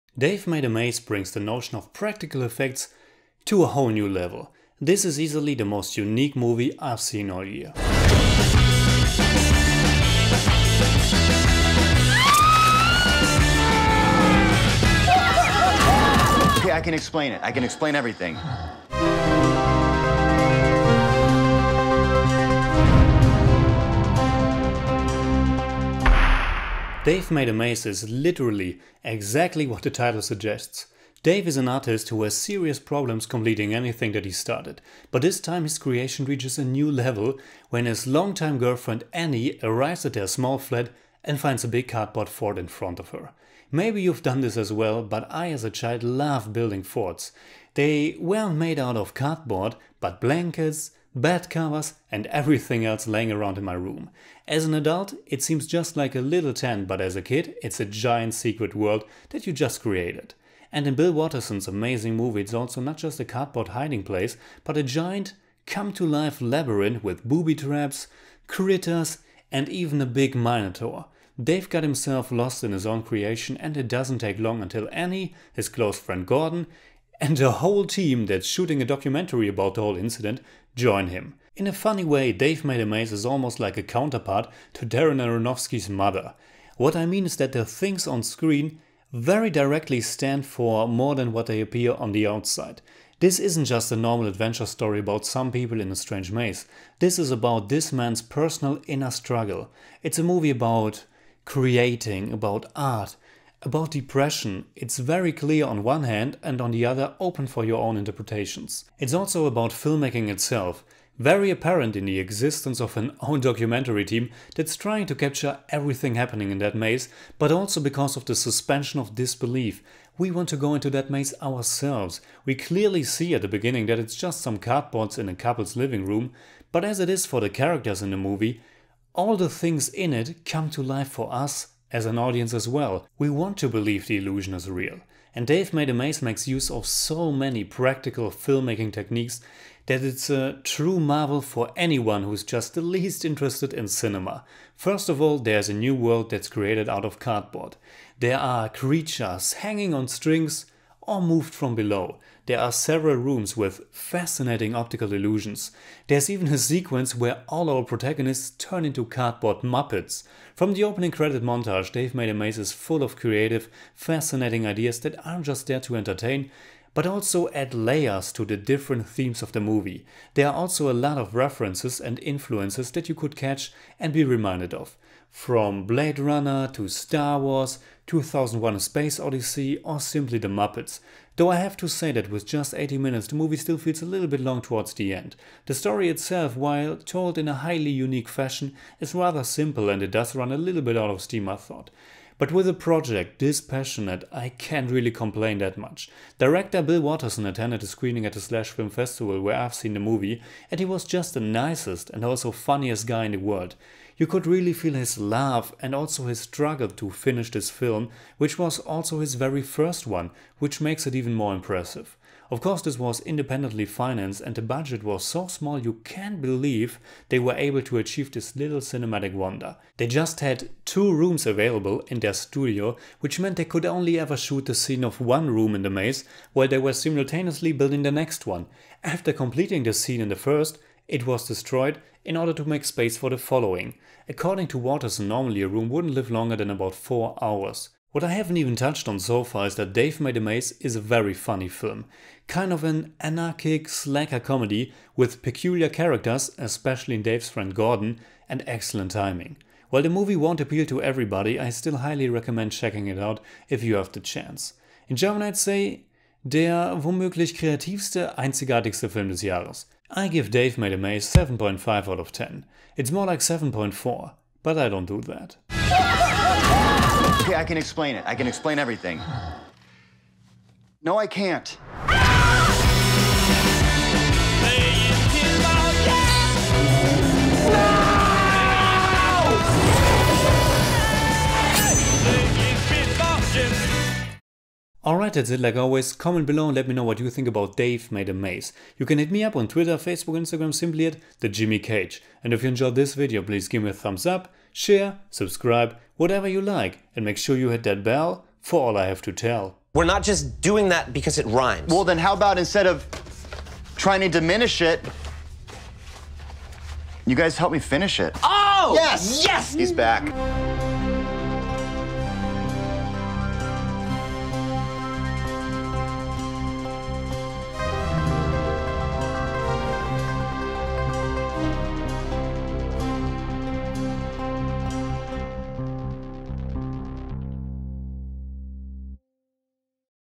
Dave Made a Maze - Movie Review
Jimmy Cage reviews DAVE MADE A MAZE (2017). Starring Meera Rohit Kumbhani, Nick Thune, Adam Busch and James Urbaniak. Directed by Bill Watterson.

Deniz the Buddymeister also just started his very own podcast about the music of film, television and video games.